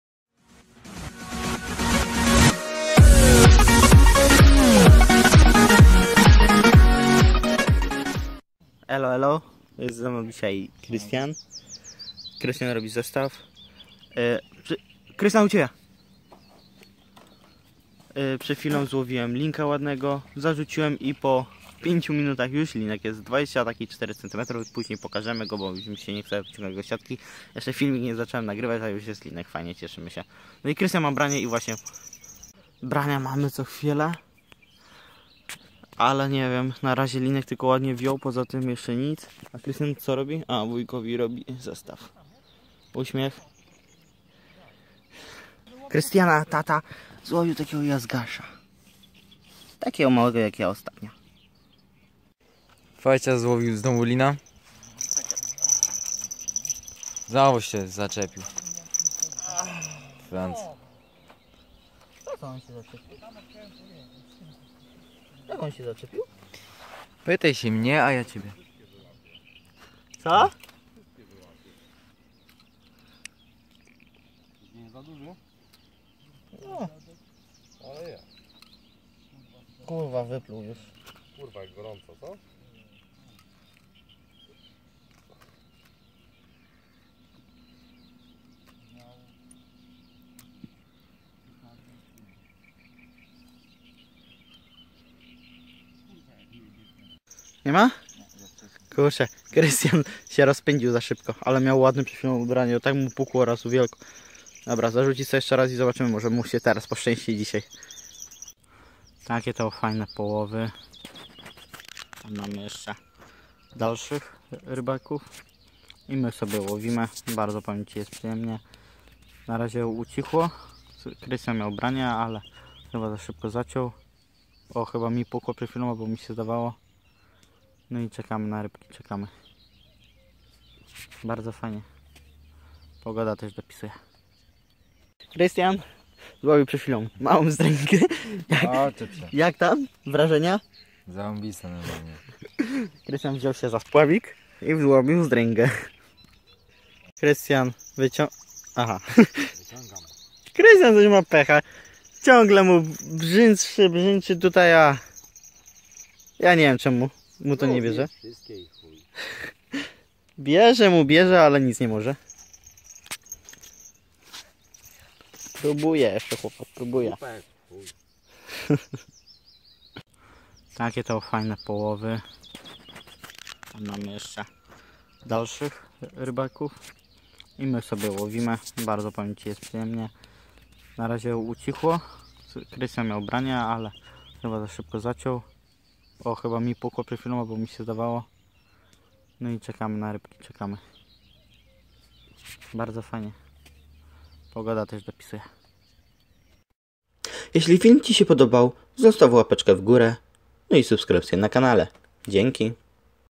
Stacha życiówka - Ogromna Ryba 2018
Film krótki i z powodu rodziny Krystiana krepowalismy się nagrywać dlatego film taki jest. Prosze wybaczyc i obiecujemy że następny film Bedzie zajebist... ! :D
# pokaz całego filmu czyli:
- pokazywanie holowania ryb
- pokaz ryb
- wypuszczenie
# pełne zakończenia
# opis łowiska
Wprowadzamy mega zmiany i staramy się od teraz wprowadzić kanał z wielkim zaangażowaniem :)
I już niedługo nocka na wielkie ryby !

🔹Zakupy wędkarskie - Szczupak75.pl ( 2018 )

 🔹Dziękuję zę was tyle już jest !
 🔹A to specjalny film dla was !!

Uwaga !!

Tu tez mozesz u szczupaka zrobic zakupy !